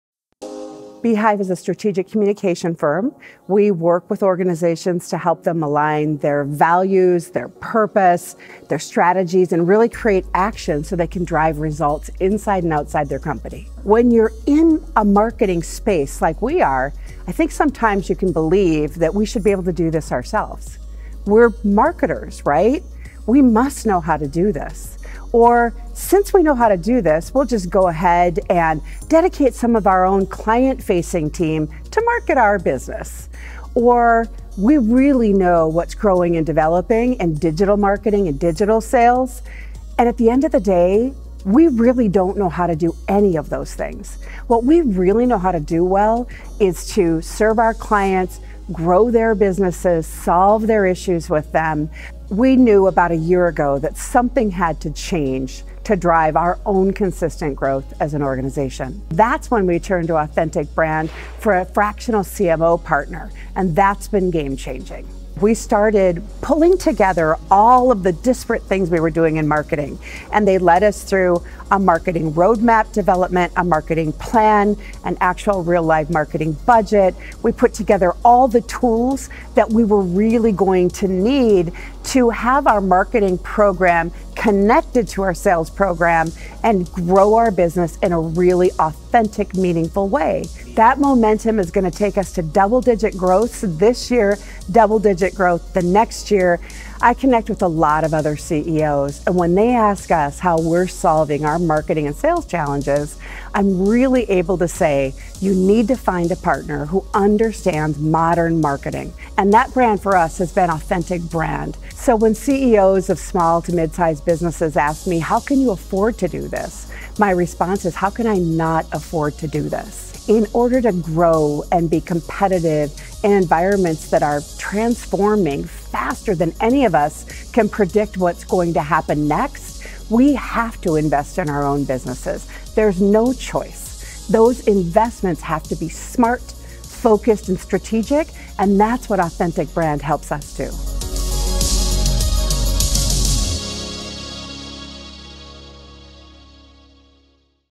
Beehive Strategic Communication - Authentic Growth™ Success Story
As a firm that helps businesses develop and refine their communication strategies, it's easy to imagine that tackling marketing for your own business would be a snap.

What Beehive Strategic Communication realized was that when everyone owns marketing, no one owns marketing. They knew they couldn't keep taking an "all hands on deck" approach. Beehive needed to get focused if they were going to be successful at marketing.

It takes tremendous humility and leadership to realize a shift like this. Fortunately, Lisa Hannum has the strength of character to realize that it's ok to seek out help when things are not working out as you intended.

Beehive Strategic Communication engaged Authentic Brand to coach them through this transformation. Ultimately, it was determined that what Beehive needed was someone who had the sole function of owing marketing. Authentic Brand was able to provide a seasoned fractional marketing leader that was perfect for what Beehive needed.

Watch this video to hear Lisa Hannum, CEO of Beehive Strategic Communication discuss their journey to marketing sophistication.